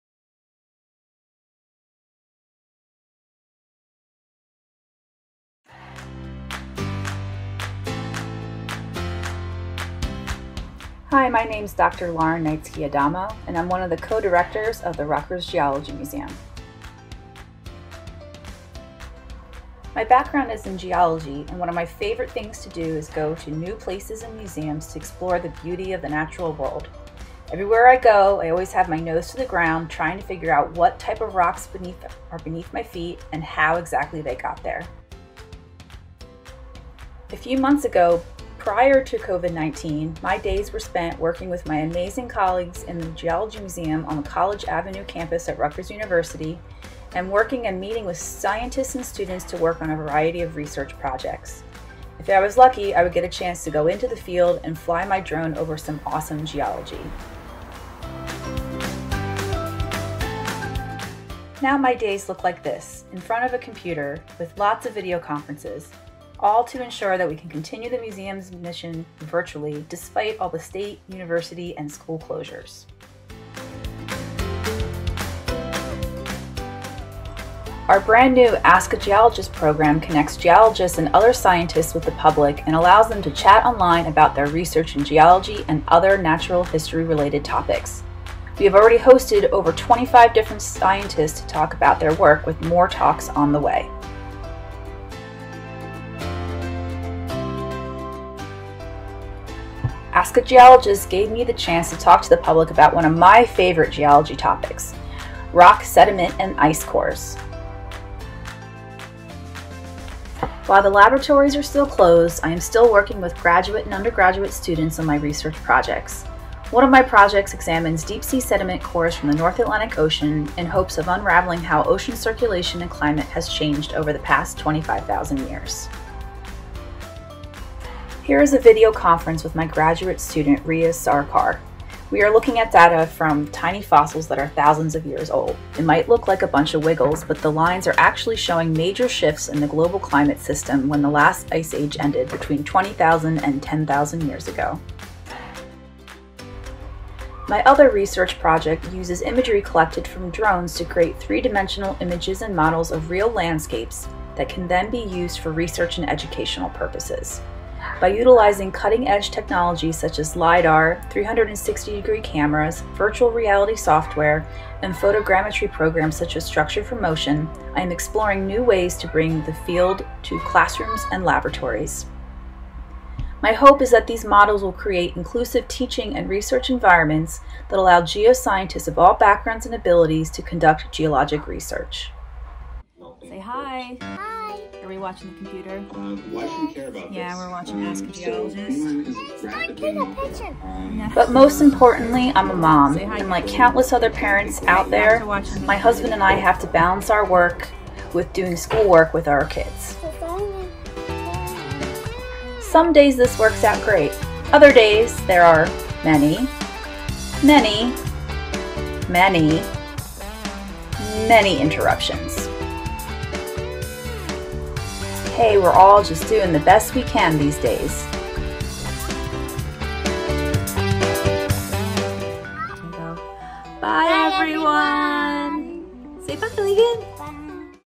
STEM Professional Lauren Adamo
Happy Wednesday! This Week's STEM Professional is Dr. Lauren Adamo! She is a Professor and the Co-Director of the Rutgers Geology Museum! Check out her video to see how she got interested in Geology, the kind of research that she does (she gets to use drones!) and what her life has been like from home!